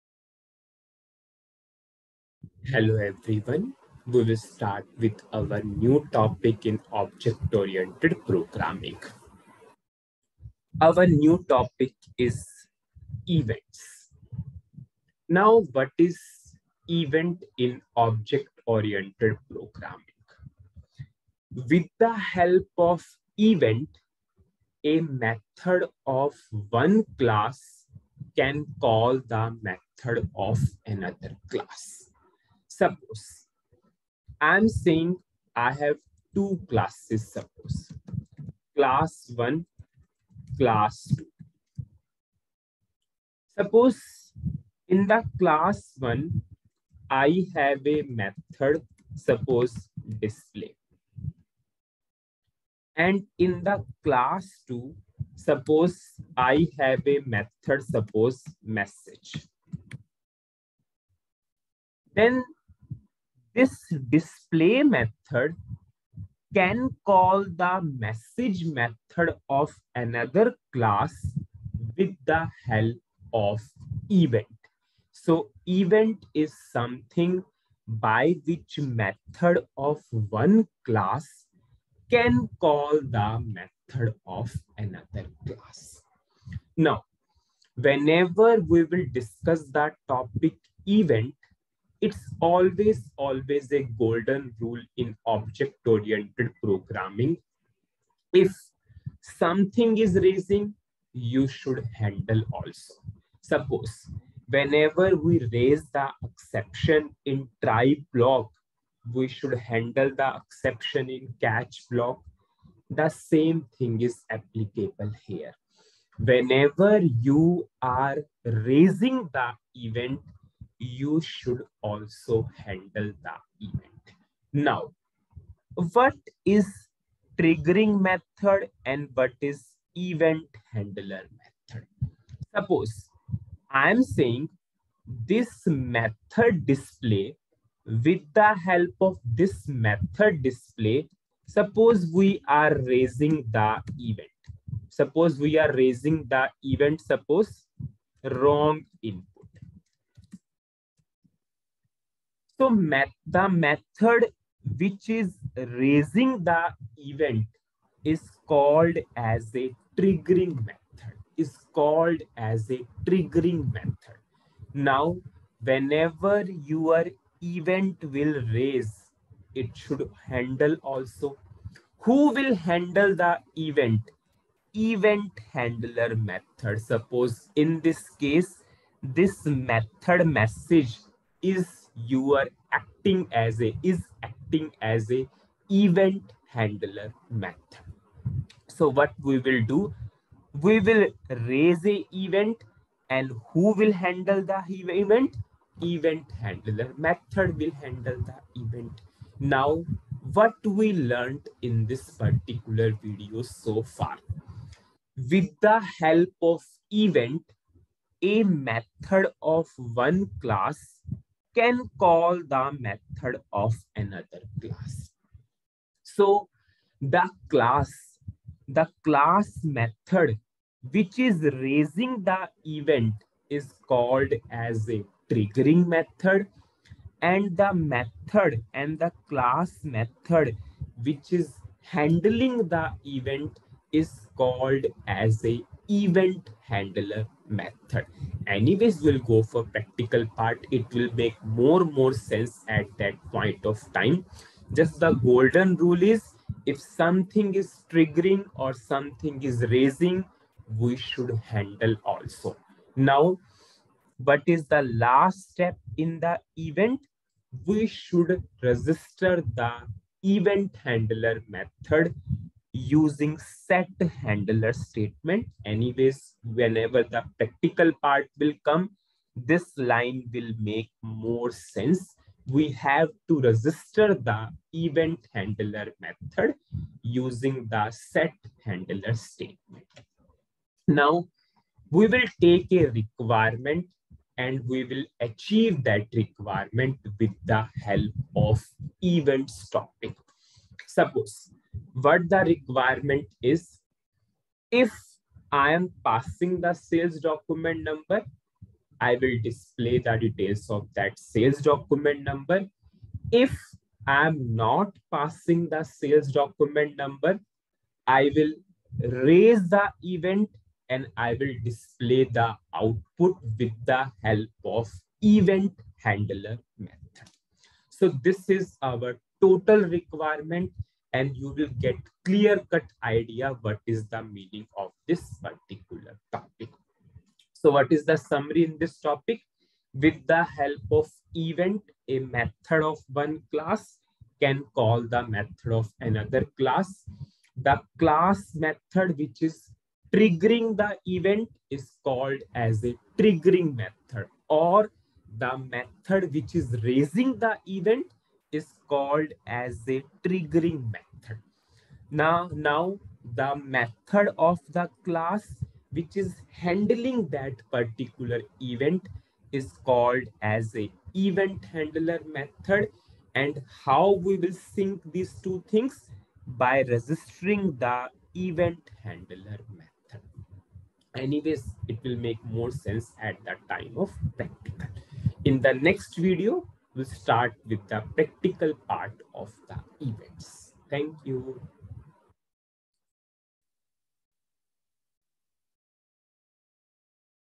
37 - ABAP OOPS - Events - Introduction
1) Meaning of Event.
2) Concept of Triggering Method.
3) Concept of Event Handler Method.